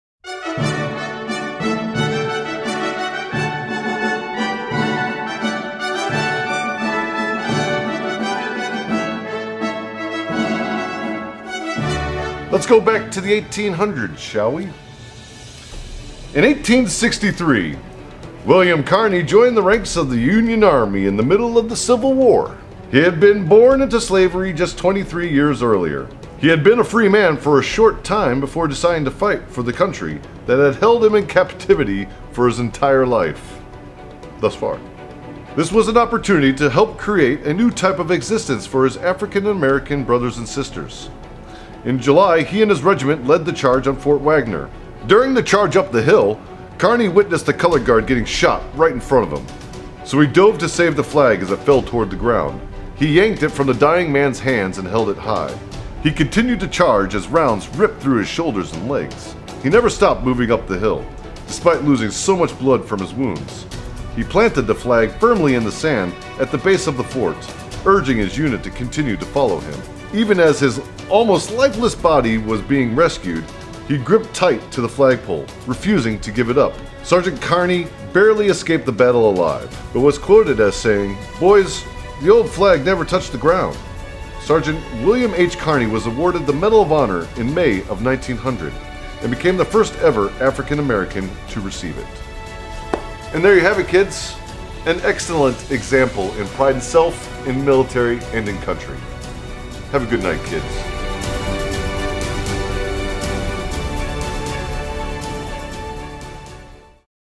Violent But True: William Carney
Season 2, Episode 18.  In the Civil War, the color guard was a dangerous but important role.  Few people executed it as well as Sgt Carney.